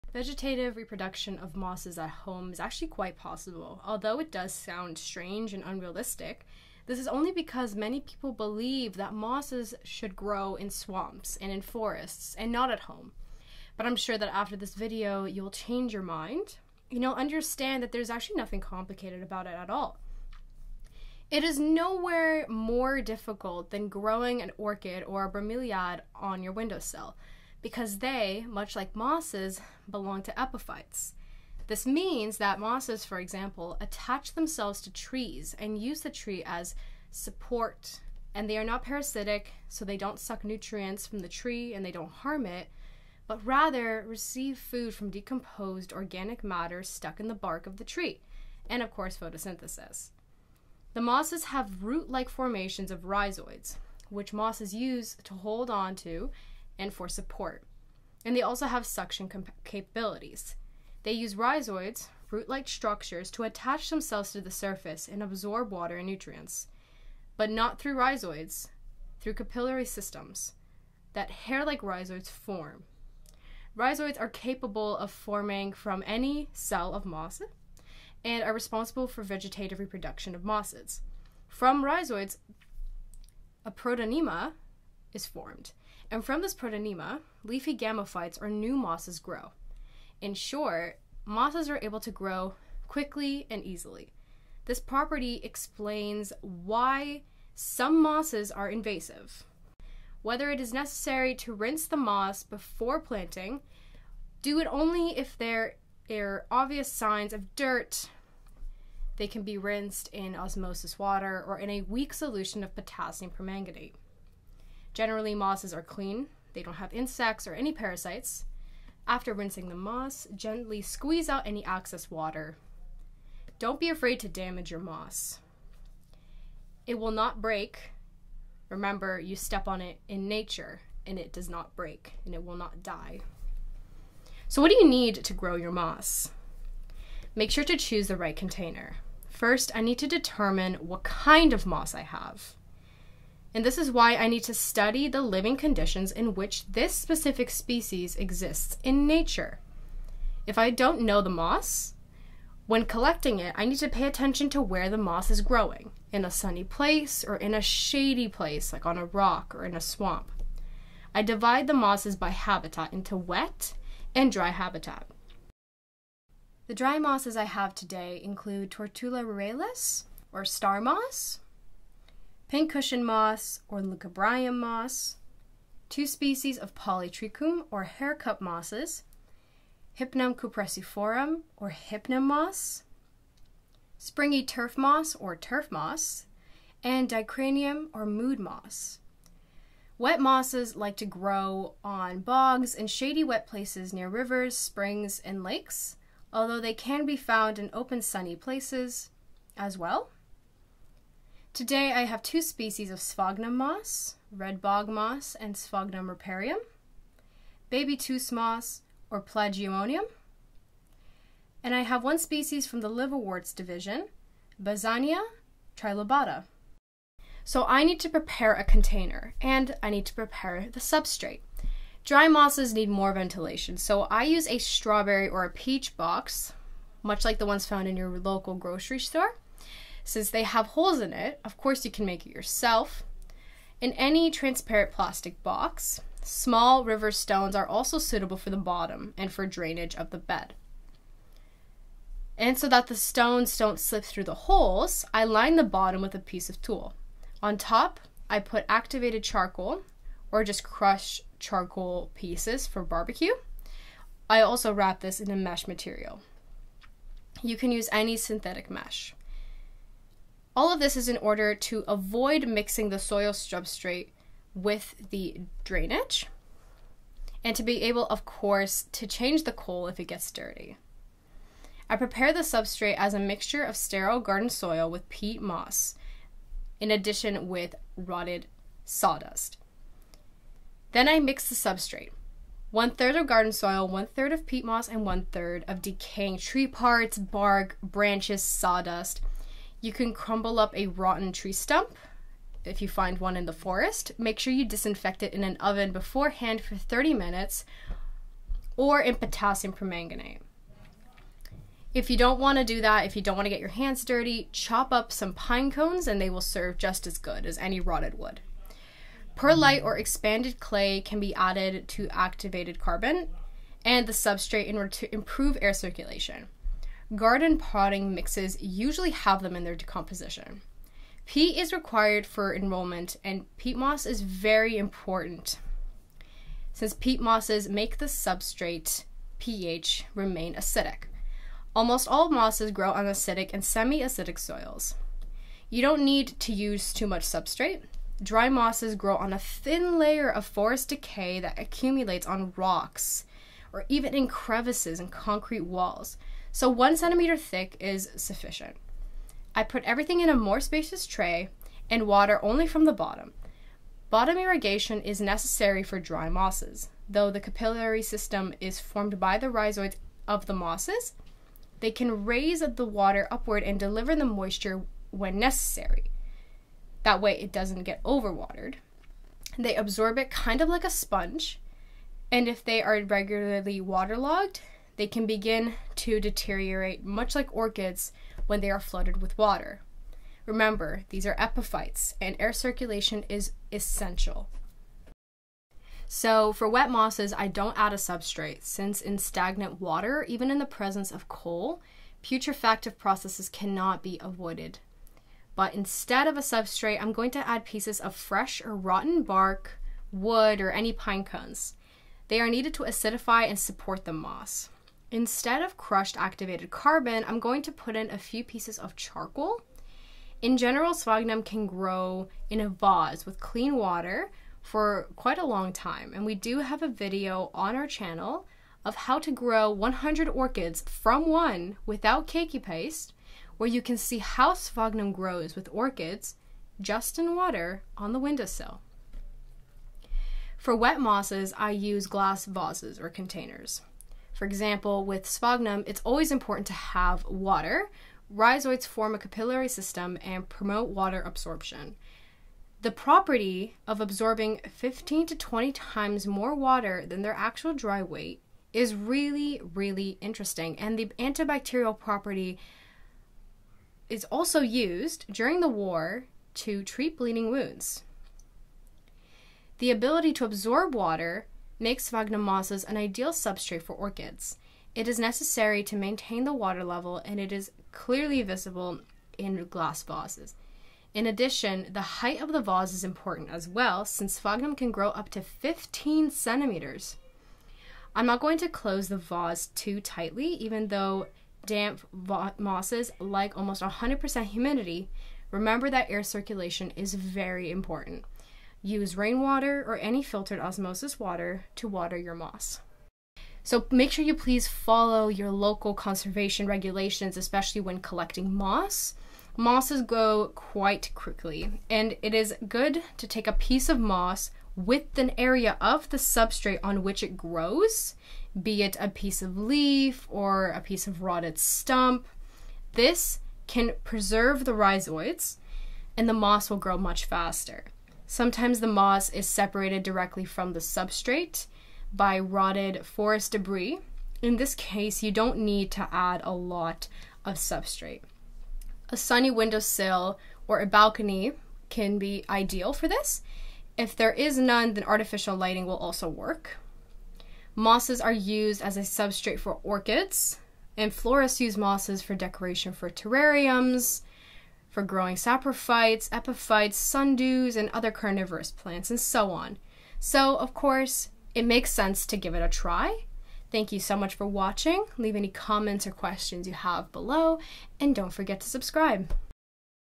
How to grow and propagate moss.
Growing mosses at home Vegetative reproduction of mosses at home is quite possible, although it sounds strange and unrealistic, only because not many people imagine how this is possible. There is a stereotype in our head that mosses should grow in swamps or in a forest, but not at home, but I am sure that after this video you will change your ideas about mosses and you will understand that there is nothing complicated about it. Not a drop is more difficult than growing an orchids or bromeliad on a windowsill, because they, like mosses, belong to epiphytes, that is, this means that mosses, for example, attaching to a tree use the tree only as a support and are not parasites and do not suck nutrients from the tree and do not harm it, but receive food from decomposed organic matter stuck in the bark of a tree and photosynthesis The mosses have root-like formations of rhizoids which mosses hold on to supports and which have a suction capacity. They use rhizoids, root like structures to attach themselves to the surface and absorb water and nutrients, but not through rhizoids, but through the capillaries game system, that hair like rhizoids form. Rhizoids are capable of forming from any cell of moss and are responsible for vegetative reproduction of mosses. From the rhizoids, a protonema is formed, and from the protonema, leafy gametophytes or new mosses grow. In short, mosses are able to grow quickly and easily vegetatively. This property explains the invasiveness of some mosses. They are even capable of crowding out your green lawn or filling up a stone path.